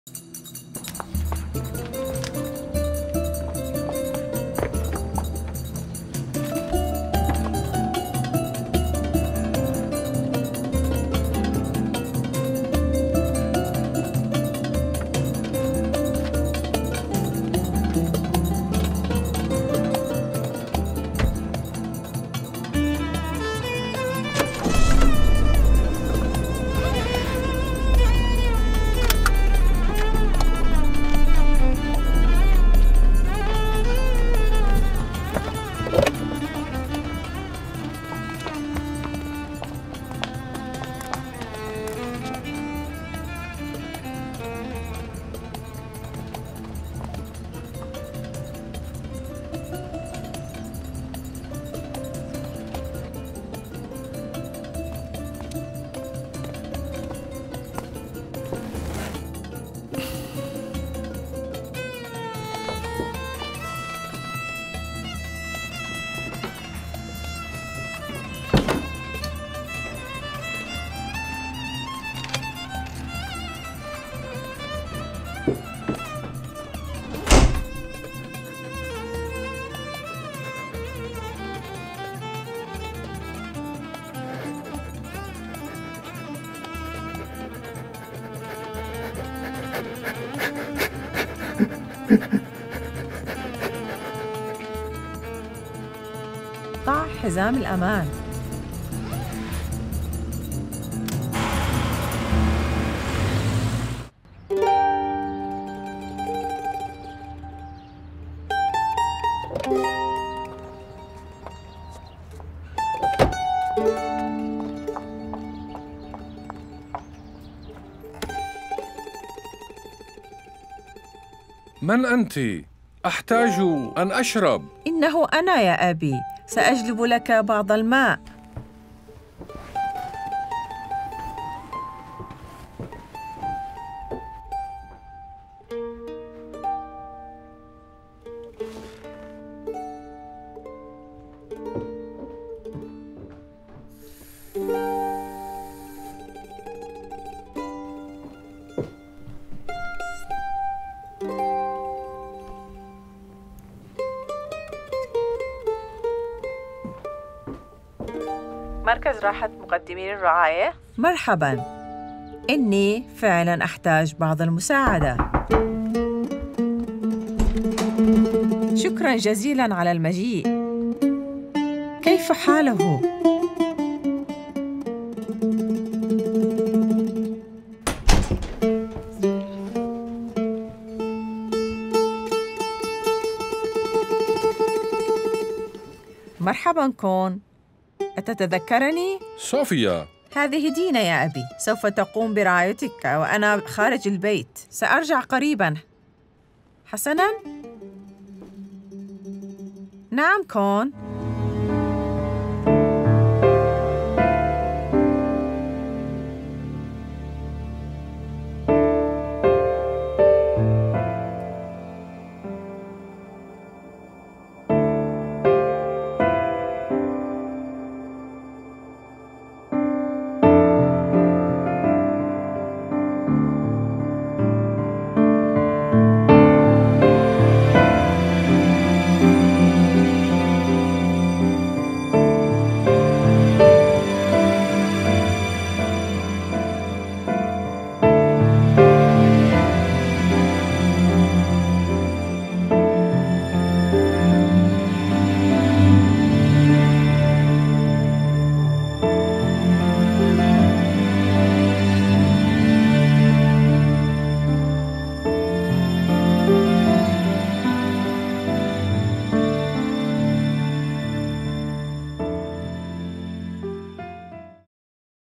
WMW 2016 Standalone Arabic KATERINA & CON
The Ethnic Communities’ Council of Victoria coordinates a statewide project delivering community education and raising awareness of elder abuse, its prevention and supports among culturally and linguistically diverse seniors in Victoria.